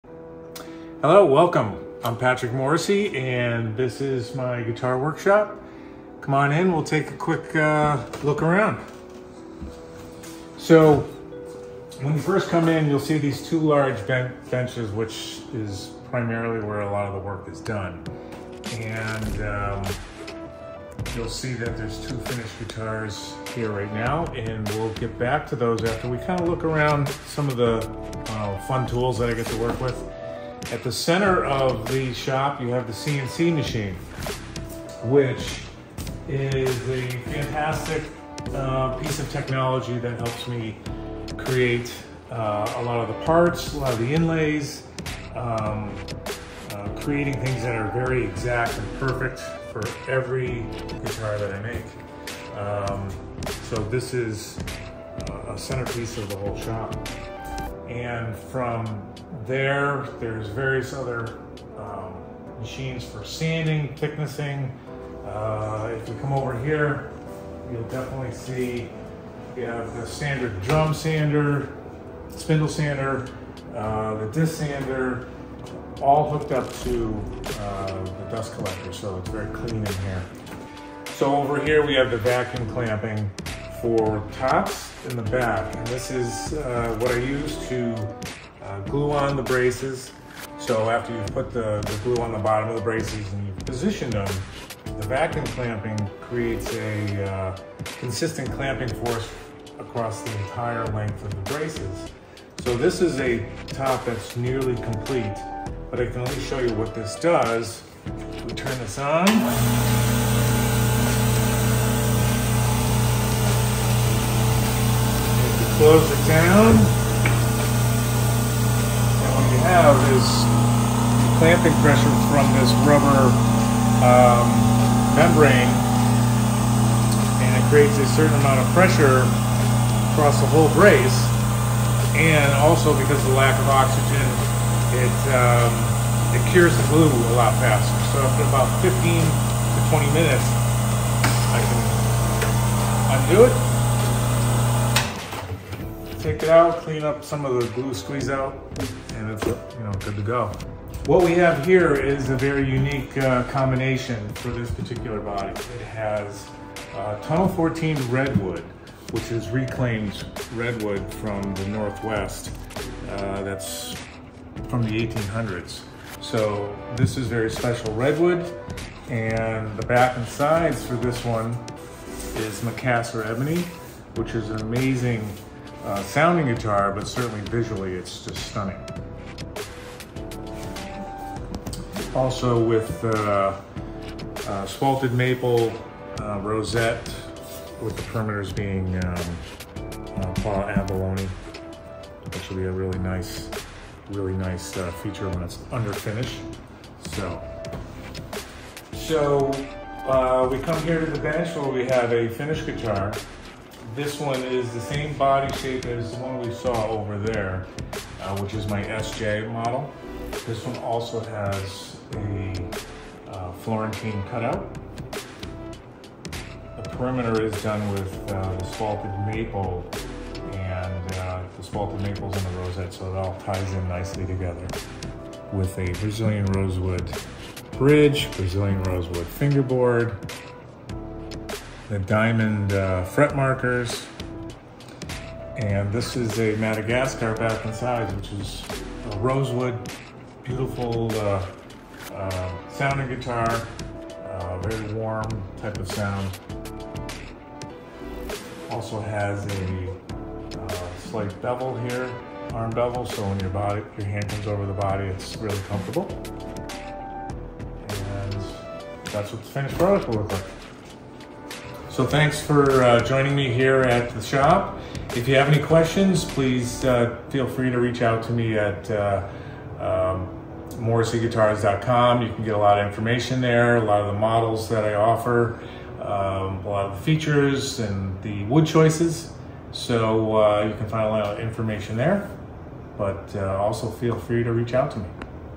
TOUR: The Morrissey Guitars workshop
Patrick Morrissey is a skilled luthier. In this video, he takes HopNews readers on behind-the-scenes tour of his workshop to show us how he builds his legendary guitars.